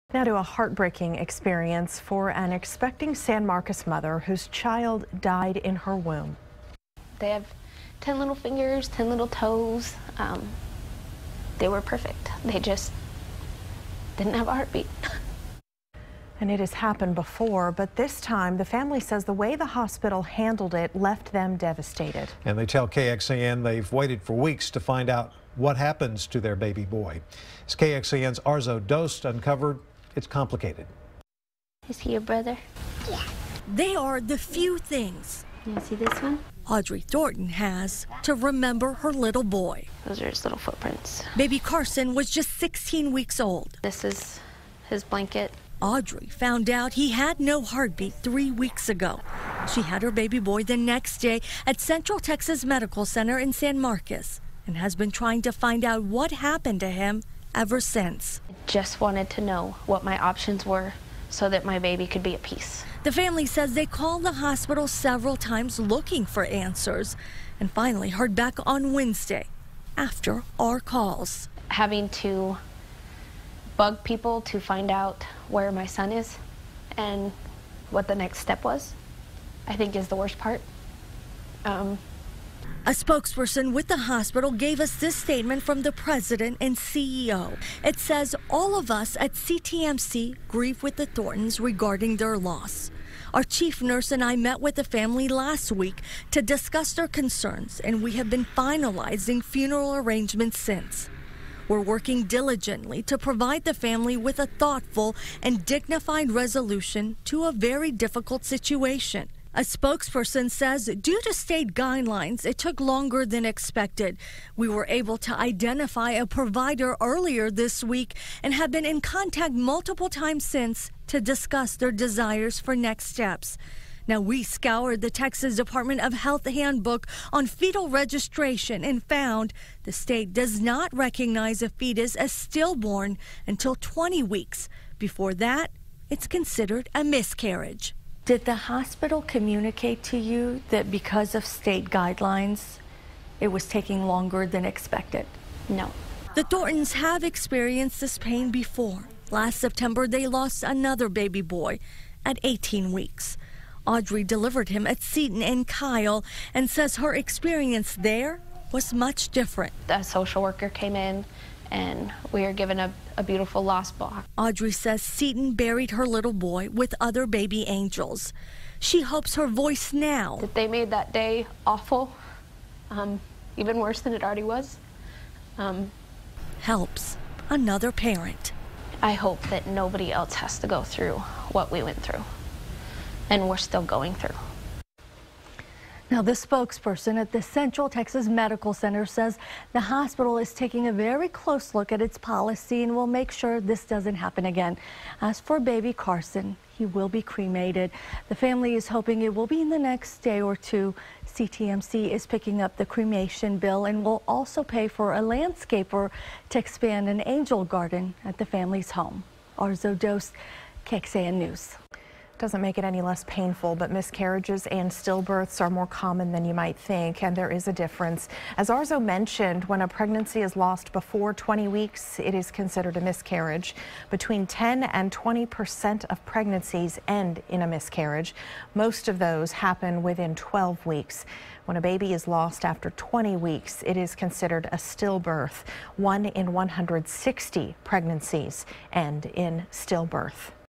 San Marcos mother fights for baby's remains
Audrey Thornton lost her baby, Karson, at just 16 weeks of pregnancy due to a miscarriage.